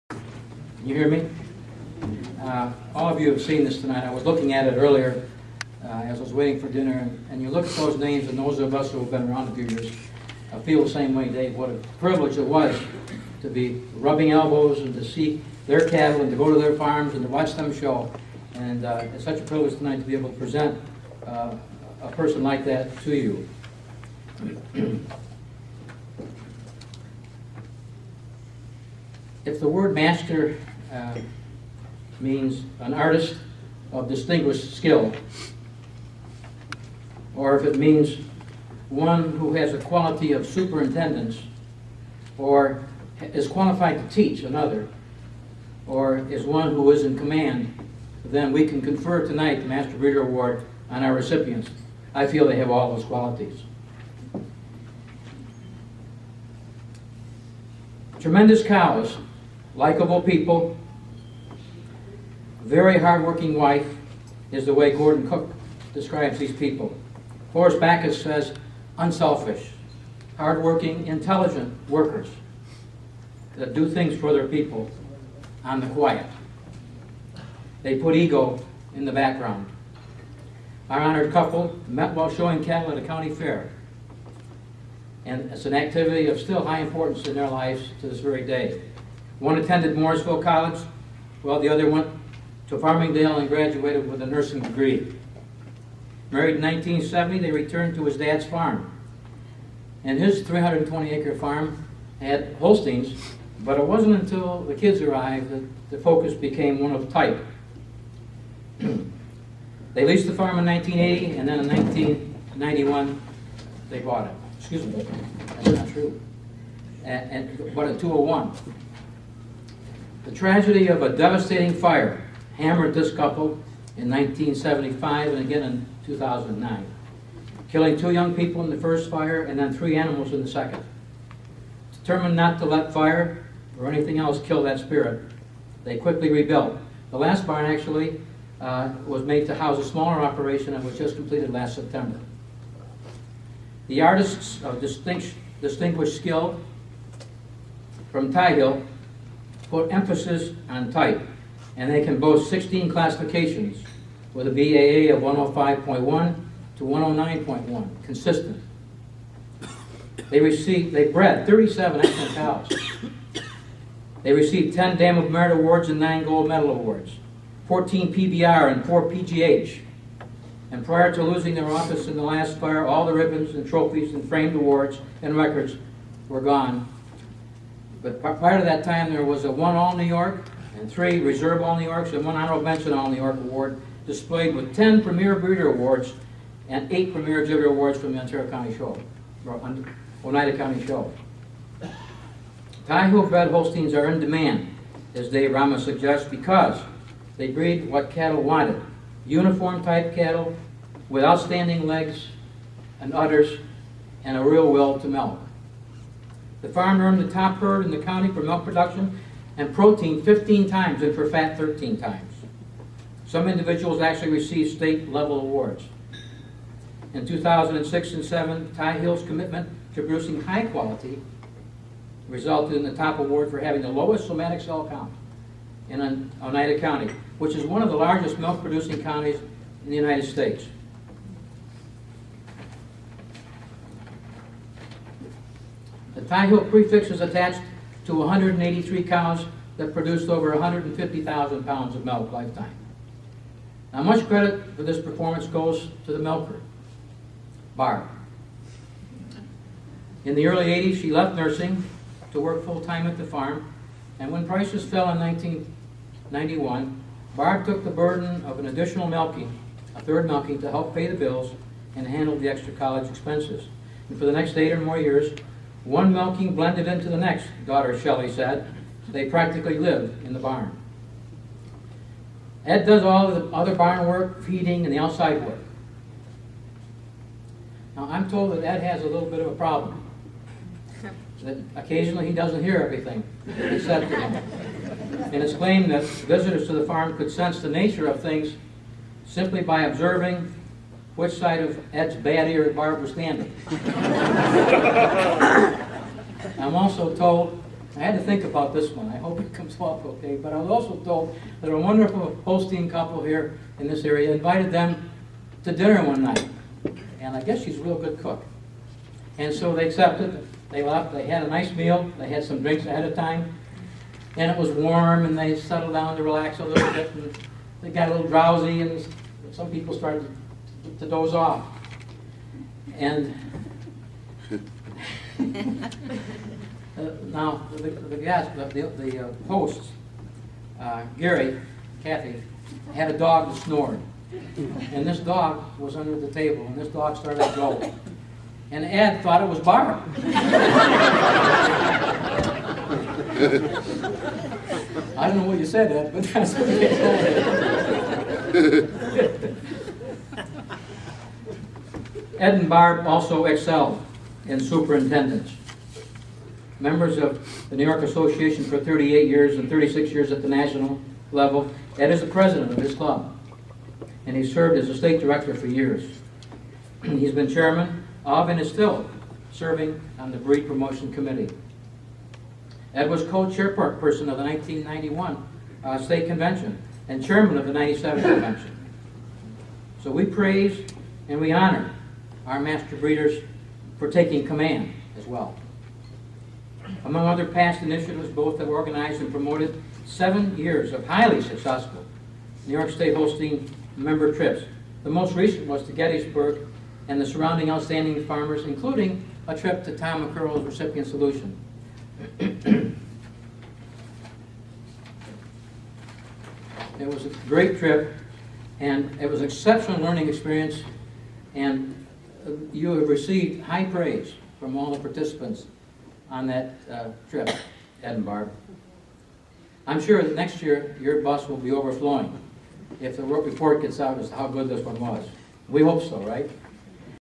2010 NY Retired Master Breeder Presentation - Part 1
The 2010 Retired Master Breeder award was presented by John Sullivan to Ed & Barb Tyler of Ty-Hill Holsteins.